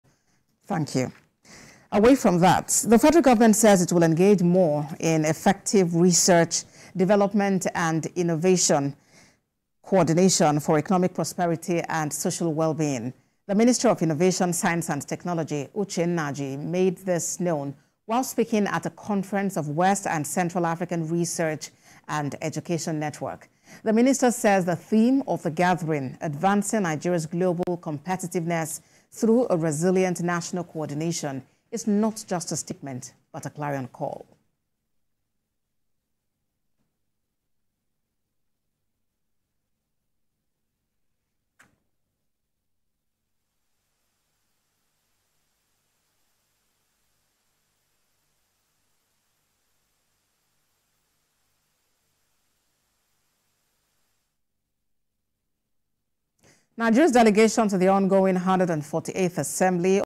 FG to Include Effective Research to Boost Sicial Well-Being
The federal government says it will engage more in effective research, development and innovation coordination for economic prosperity and social well-being.
The Minister of Innovation, Science and Technology, Uche Nnaja made this known while speaking at a conference of west and central African research and education network.
The minister says the theme of the gathering, “Advancing Nigeria’s Global Competitiveness Through a Resilient National Coordination,” is not just a statement but a clarion call.